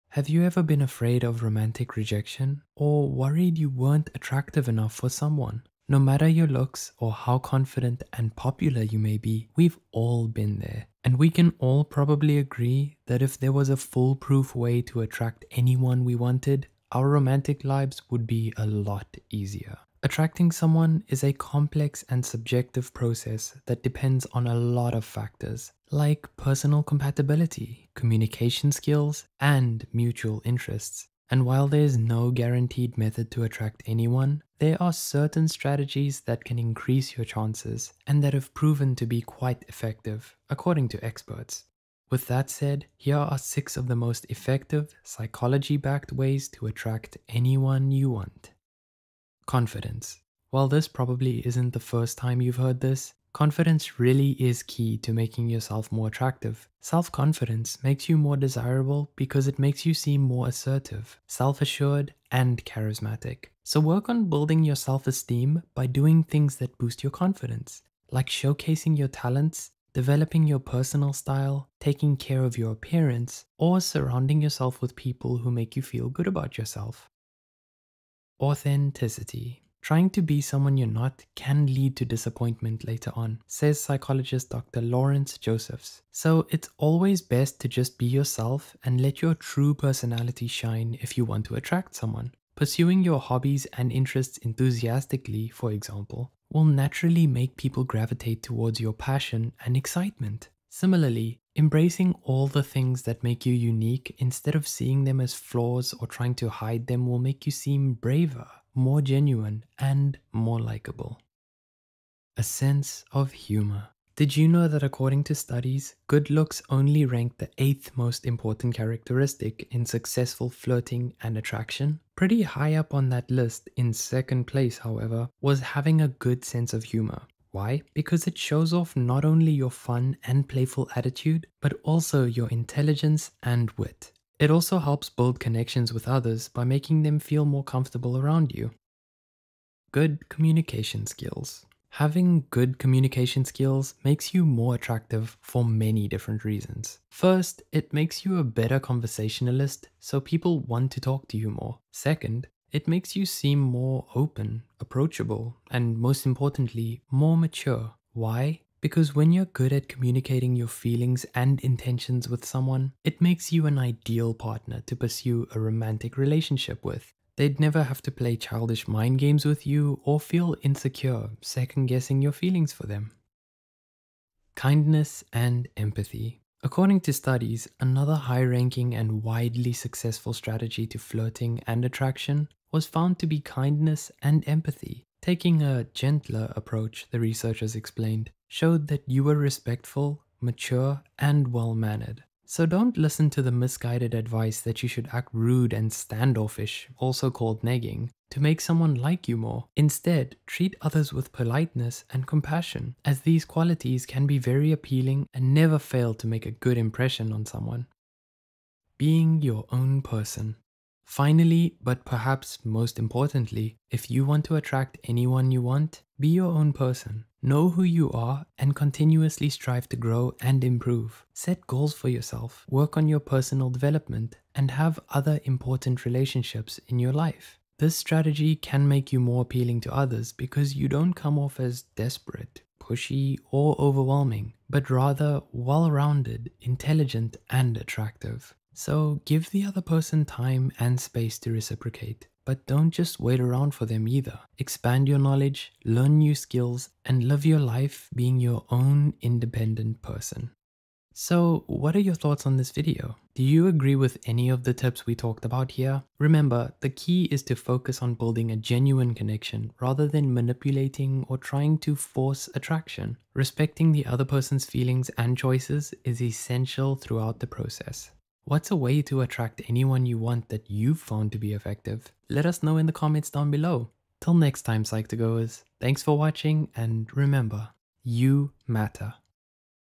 6 Most Effective Ways To Attract Anyone You Want
Want to know how to attract anyone? Whether you're wondering how to attract your crush, seeking to enhance your dating skills, or wanting to create a stronger connection in your relationship, this video is your go-to guide for relationship advice and mastering the art of attraction.

Join us on this journey of attraction mastery, filled with practical tips and actionable advice on love, dating, and relationships. Whether you're crushing, dating, or in a relationship, these insights will help you navigate the complex world of attraction.

Writer: Chloe Avenasa
Editor: Caitlin McColl & Kelly Soong
Script Manager: Kelly Soong

References:
Apostolou, M., & Christoforou, C. (2020). The art of flirting: What are the traits that make it effective? Personality and Individual Differences, 158, 109866.
Ben-Zeév, A. (2021, Mar 23). What Makes a Good Flirt? Psychology Today.
Josephs, L. (2020, Oct 3). How to Make Yourself More Attractive. Psychology Today.
Wu, K. (2017, Feb 14). Love, Actually: The science behind lust, attraction, and companionship. Harvard University. sitn.hms.harvard.edu/flash/2017/love-actually-science-behind-lust-attraction-companionship/